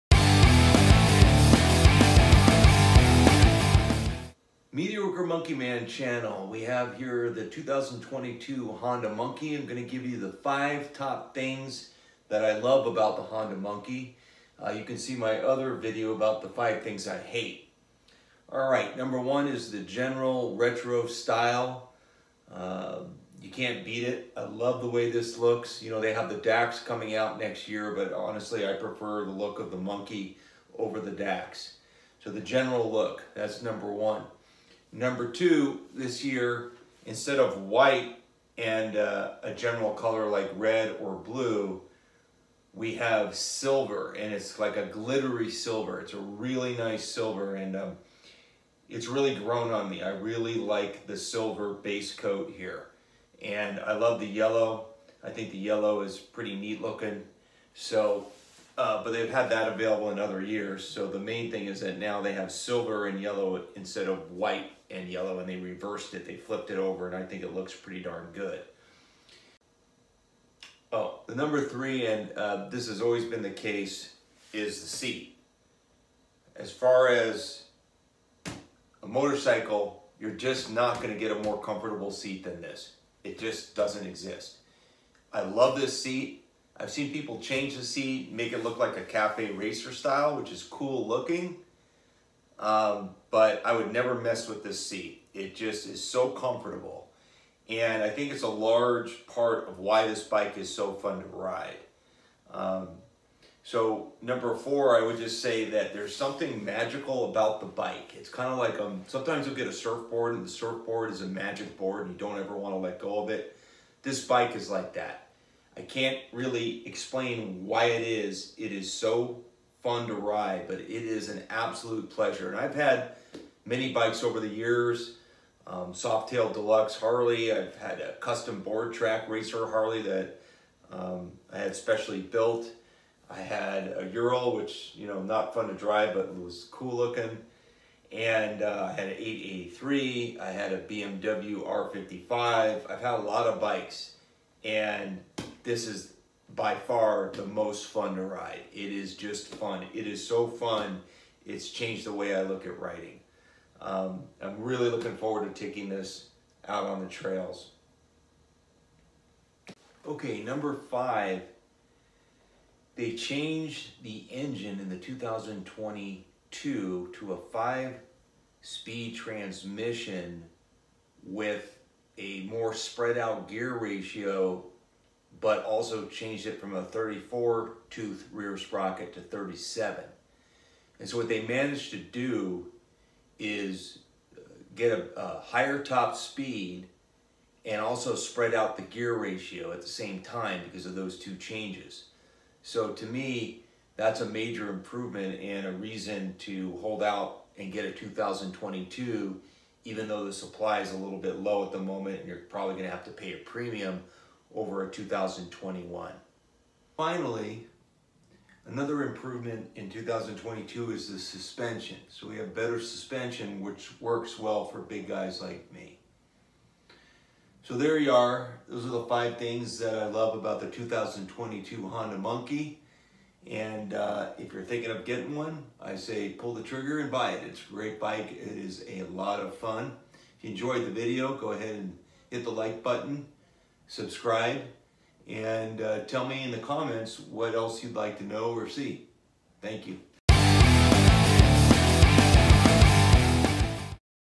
Stroking the 2022 Honda Monkey 5 Things I Love About it
Mediocre￼ Monkey man reviews the top five things he loves about the 2022 Honda Monkey